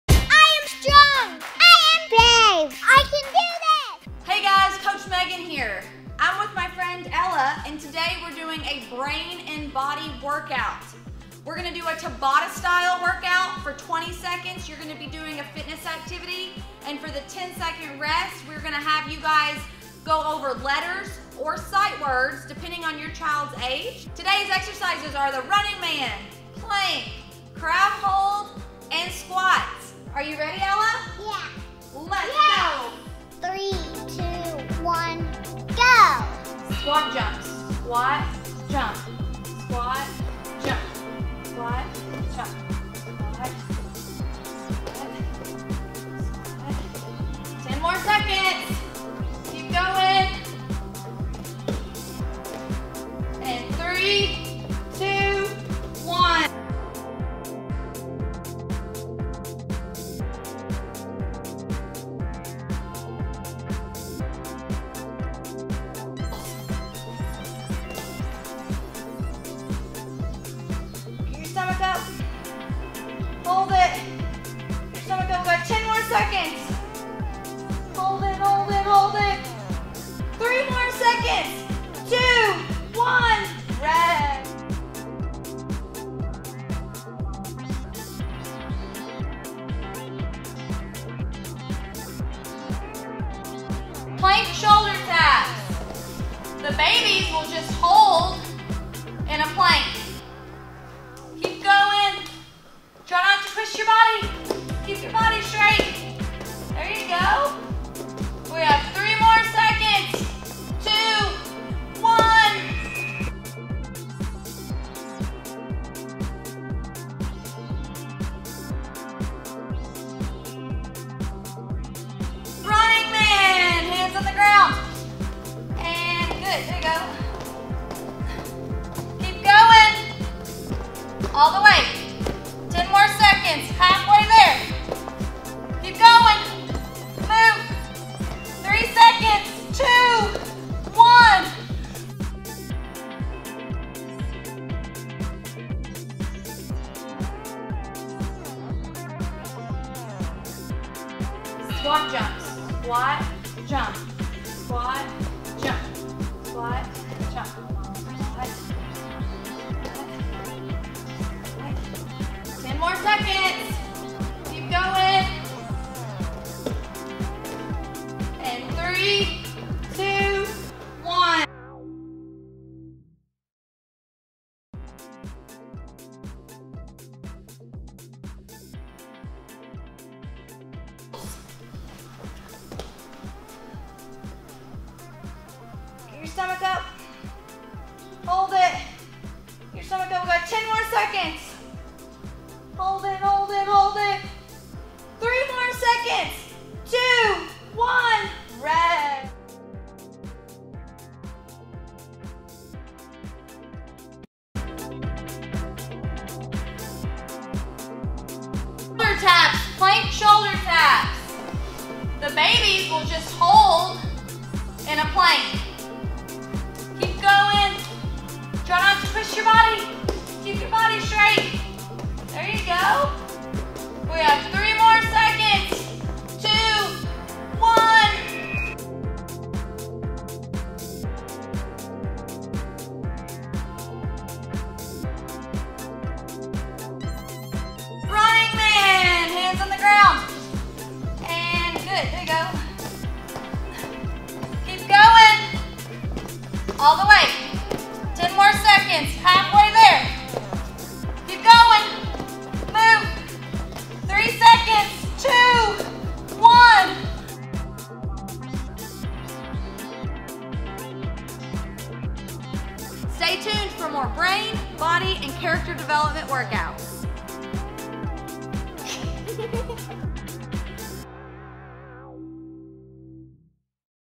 Parents and Teachers! Try this Brain and Body Workout
Teachers and Parents! Check out this quick Brain and Body exercise that you can do at home or in the CLASSROOM with your little ones!! Let's get their brains and bodies moving so they will be ready for more learning!!